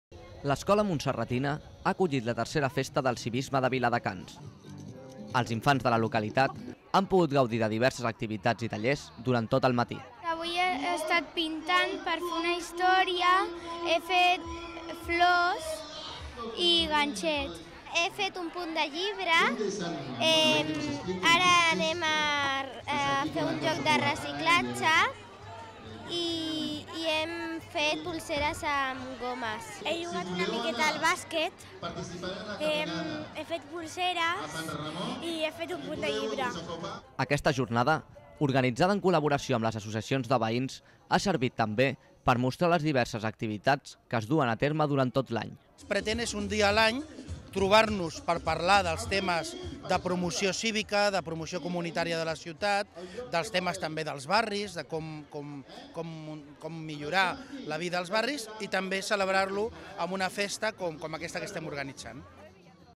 Viladecans celebra la 3a Festa del civisme a l’Escola Montserratina
L’Escola Montserratina ha acollit la tercera festa del civisme de Viladecans. Els infants de la localitat han pogut gaudir de diverses activitats i tallers durant tot el matí.

Aquesta jornada organitzada en col·laboració amb les associacions de veïns, ha servit també per mostrar les diverses activitats que es duen a terme durant tot l’any.